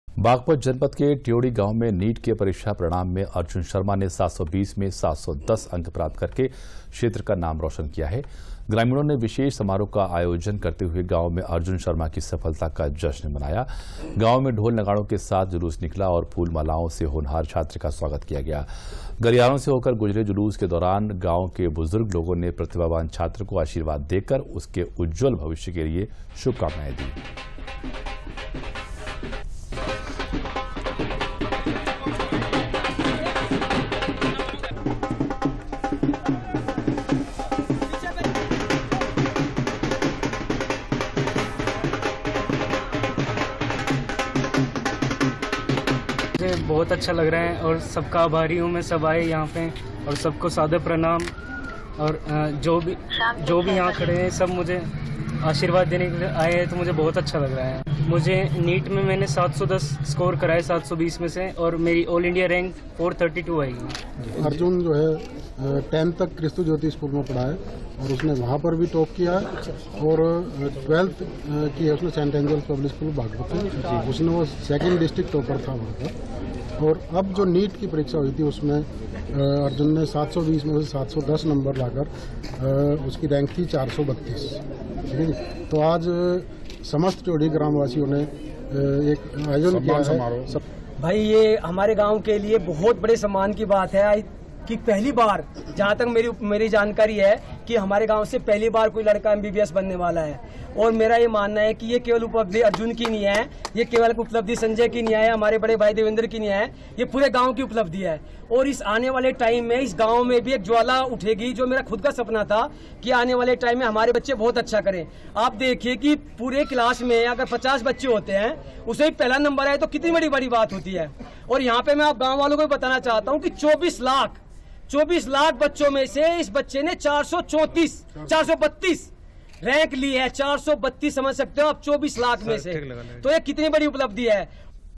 BAAGPAT : नीट के परीक्षा परिणाम में अर्जुन शर्मा ने 720 में से 710 अंक प्राप्त कर मारी बाजी
बागपत जनपद के ट्योढ़ी गांव में नीट के परीक्षा परिणाम में अर्जुन शर्मा ने 720 में से 710 अंक प्राप्त करके क्षेत्र का नाम रोशन किया है। ग्रामीणों ने विशेष समारोह का आयाेजन करते हुए गांव में अर्जुन शर्मा की सफलता का जश्न मनाया। गांव में ढोल-नगाड़ों के साथ जुलूस निकाला और फूल मालाओं से होनहार छात्र का स्वागत किया। गलियों से होकर गुजरे जुलूस के दौरान गांव के बुजुर्ग लोगों ने प्रतिभावान छात्र को आर्शीवाद देकर उसके उज्जवल भविष्य के लिए शुभकामनाएं दी।